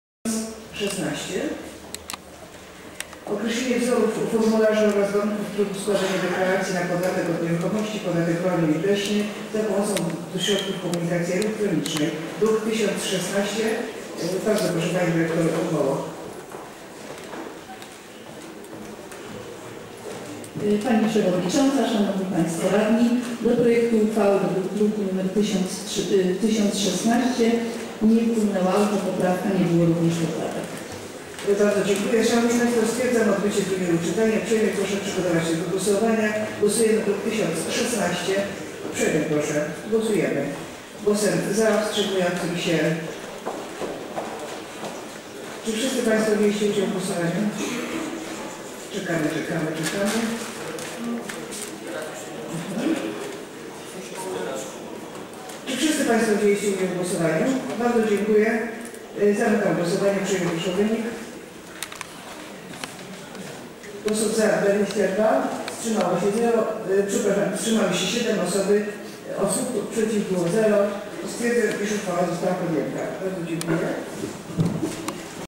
RMK_05-12-2012_05 Wzory formularzy, warunki i tryb składania deklaracji podatkowych
Sesja LXII Rady Miasta Krakowa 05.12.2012

Określenie wzorów formularzy oraz warunków i trybu składania deklaracji na podatek od nieruchomości, podatek rolny i podatek leśny za pomocą środków komunikacji elektronicznej
Projekt Prezydenta Miasta Krakowa -- druk nr 1016.